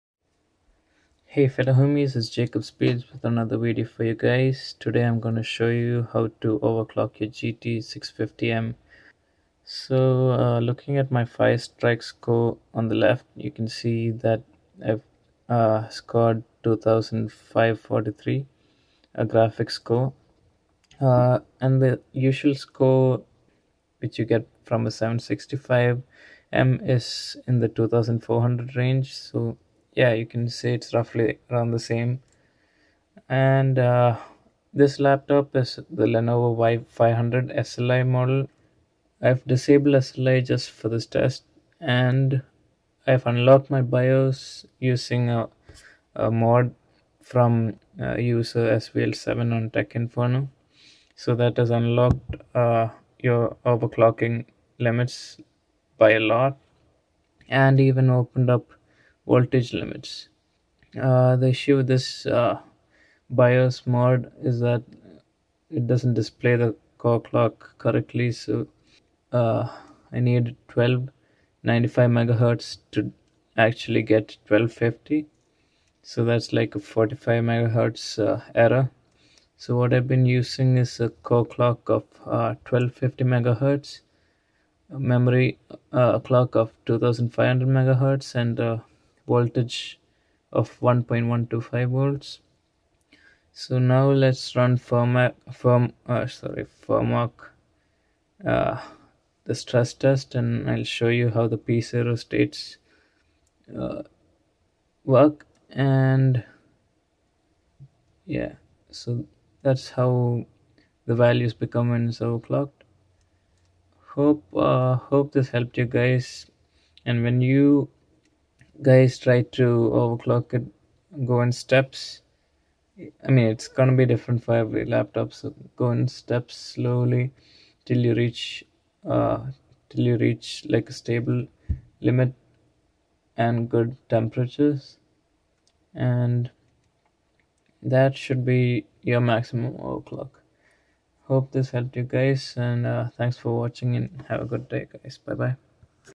Overclocking a Nvidia GT650m to GT765m levels
How I overclocked my Lenovo y500 with a gt650m to levels of a gt 765m. Temps stayed below 85C Might be able to push it higher but I'm not comfortable lol.